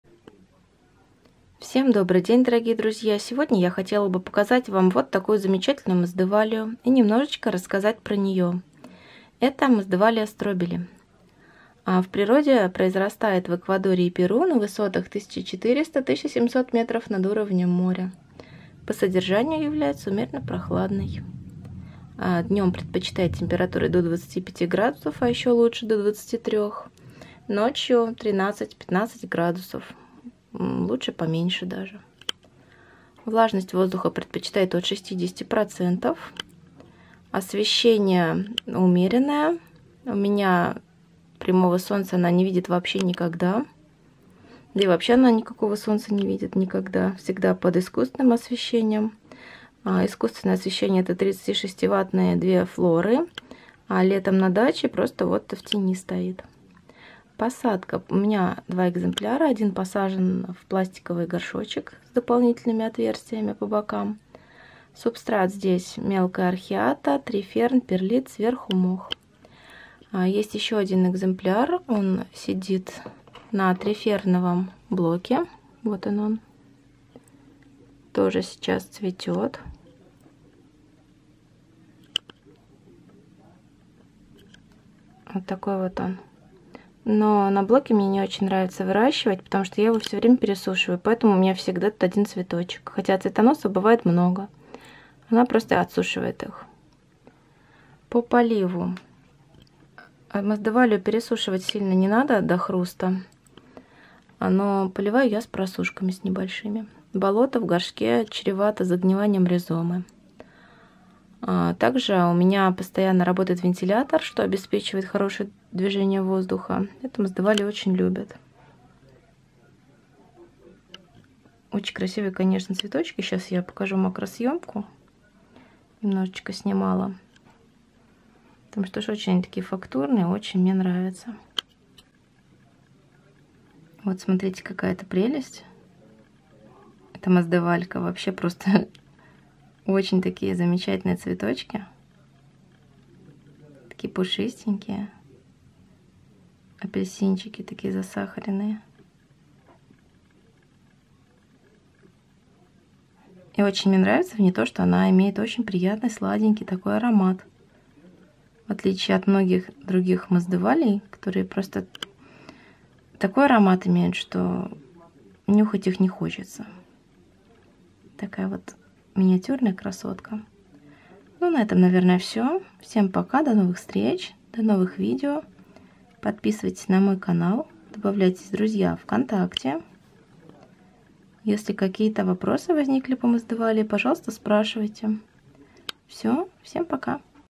Masdevallia strobelii
В этом видео покажу миниатюрную и очень красивую орхидею- Masdevallia strobelii.